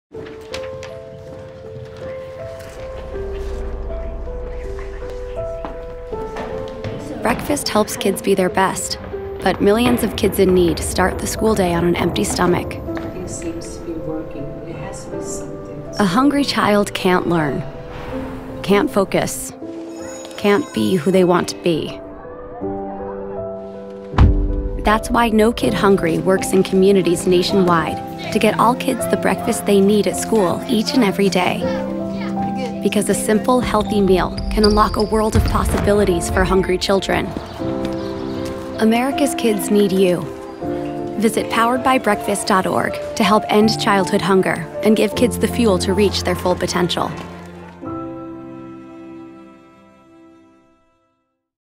No Kid Hungry’s Powered by Breakfast 2020
Millions of kids go to school with an empty stomach, making it hard to learn. This is why we’ve partnered with No Kid Hungry to expand school breakfast programs where there is the greatest need. In 2019 alone, our support helped ensure 13 million more meals are being served each year to help kids be their best. (Copyright: No Kid Hungry)